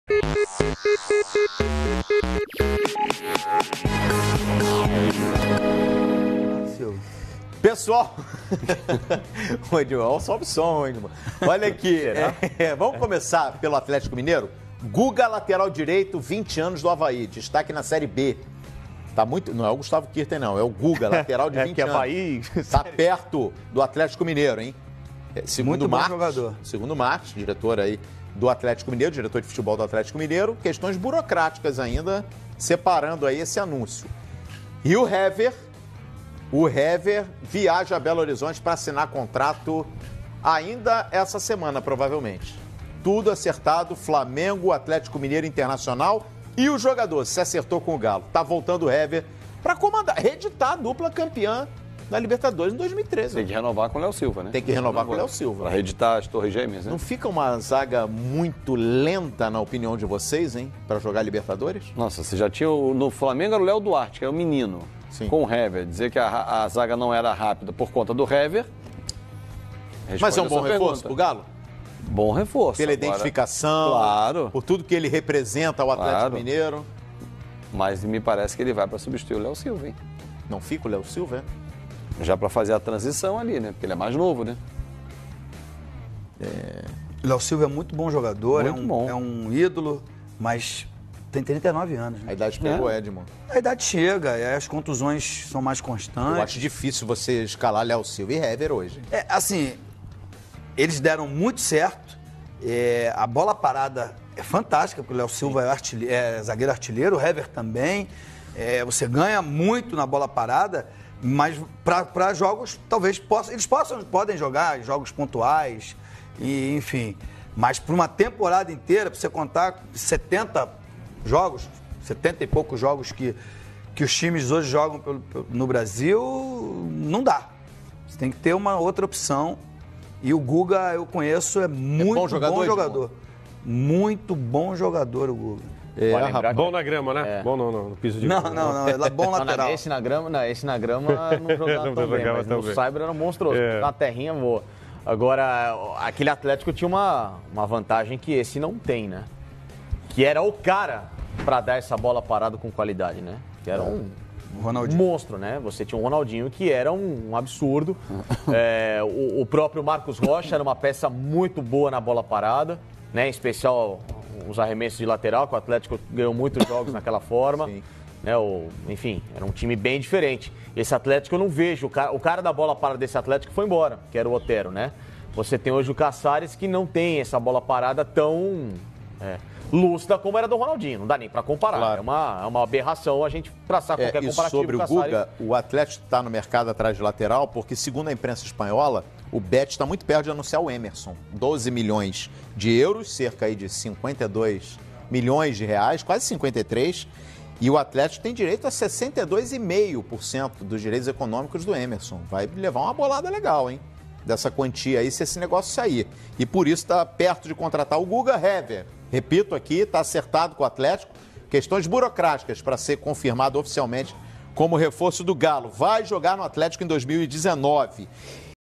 MERCADO DA BOLA: Atlético Mg estaria perto de anunciar lateral é dupla de zaga campeã em 2013;
Além disso o time mineiro estaria muito próximo de anunciar o lateral destaque do Avaí na Série B de 2018..
Atlético-Mg estaria perto de reeditar dupla de zaga campeã da Libertadores em 2013;